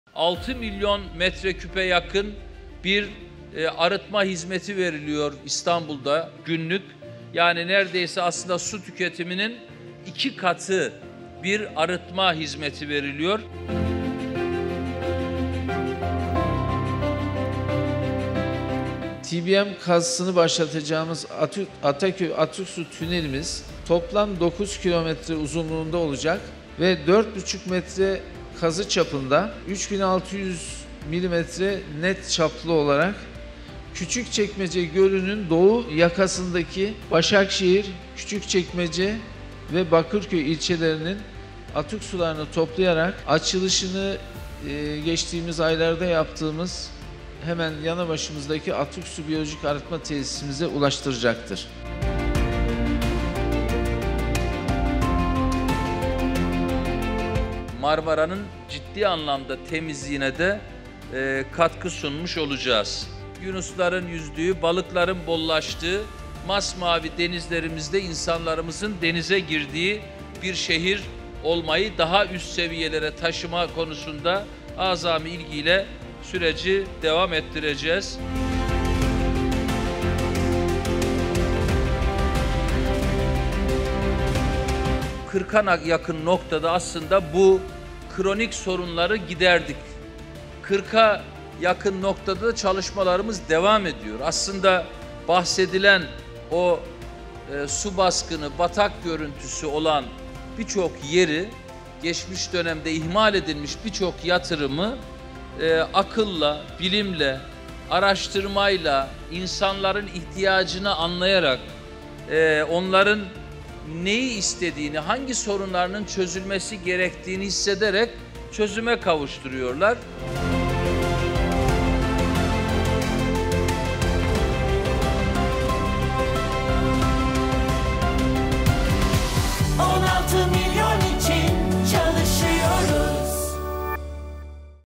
"Geçmişte ihmal edilmiş birçok yatırımı akılla, bilimle, araştırmayla çözüme kavuşturuyoruz.
Başkan Ekrem İmamoğlu, Marmara Denizi’ndeki kirlenmenin önüne geçecek en önemli projelerden biri olan Ataköy Atık Su Tüneli inşaatını başlattı.

ayrıca İBBTV’yi diğer mecralarda da takip etmek için:
Makaleler, programlar ve canlı yayın: